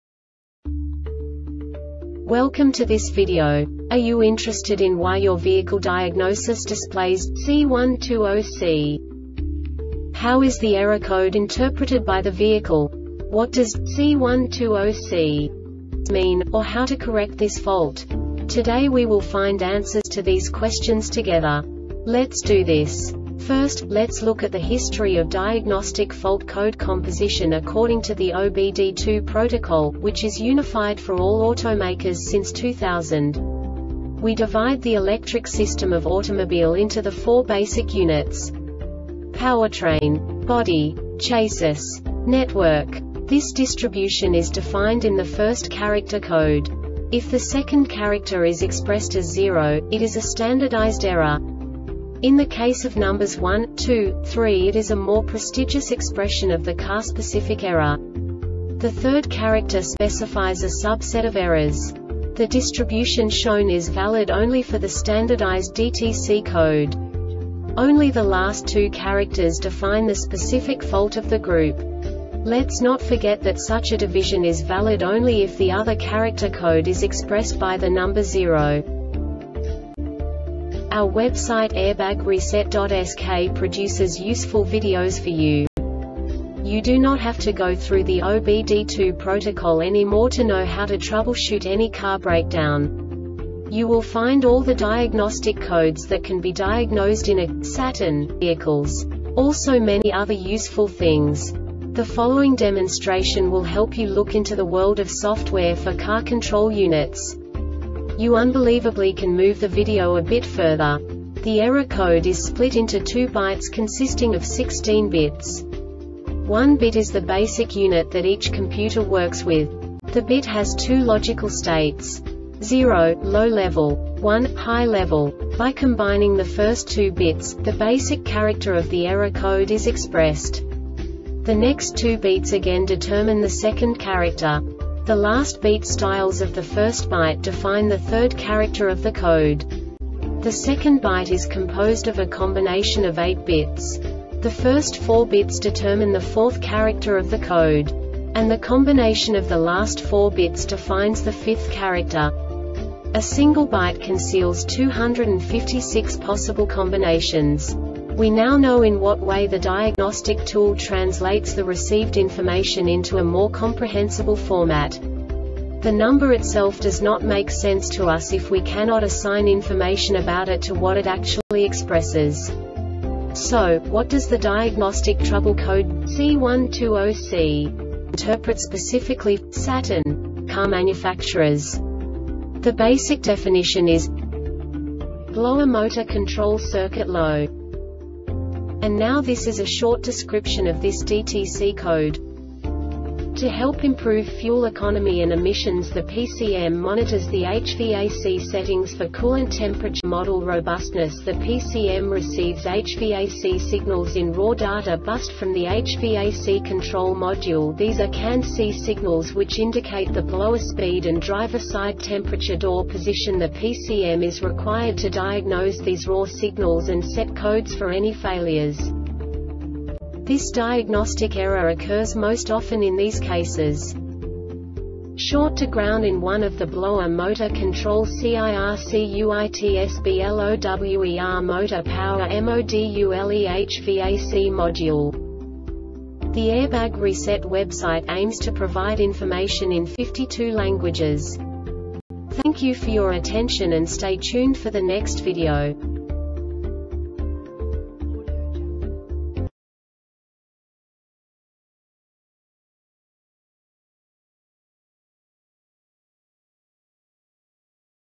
DTC Saturn C120C Short Explanation
Contents:
0:21 Basic DTC analysis according to OBD2 protocol standard.
1:48 Insight into programming
2:58 A diagnostically understandable description of the Diagnostic Trouble Code
3:05 Basic error definition

Make:
Saturn
Code:
C120C
Definition:
Blower Motor Control Circuit Low
Description:
To help improve fuel economy and emissions the PCM monitors the HVAC settings for Coolant Temperature Model robustness. The PCM receives HVAC signals in raw data bussed from the HVAC Control Module. These are CAN C signals which indicate the Blower Speed and Driver Side Temperature Door Position. The PCM is required to diagnose these raw signals and set Codes for any failures.
Cause:
SHORT TO GROUND IN ONE OF THE BLOWER MOTOR CONTROL CIRCUITSBLOWER MOTOR POWER MODULEHVAC MODULE